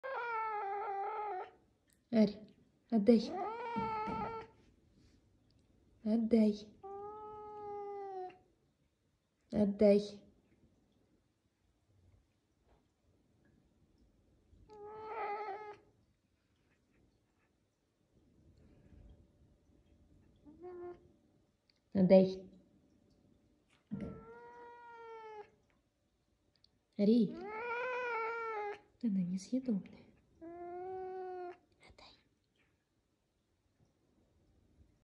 Playful caracal cat does not share toys | ariel.caracat
This is Arielka's favourite toy, she chews it really hard! I think we need a new one :)